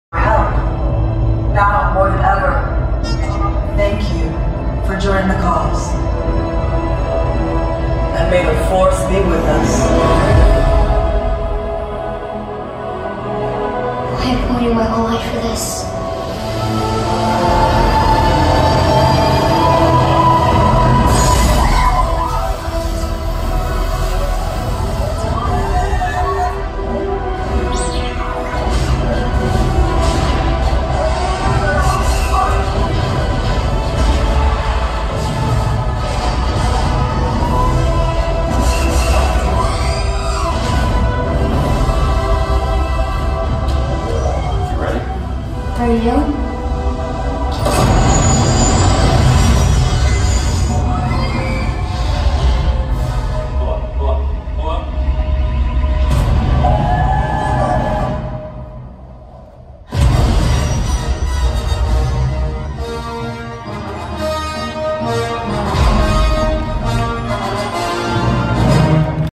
New Footage of The Rise Of The Resistance At D23
NEW footage for the ride RiseOfTheResistance at GalaxysEdge at D23Expo in Anaheim! Check it out!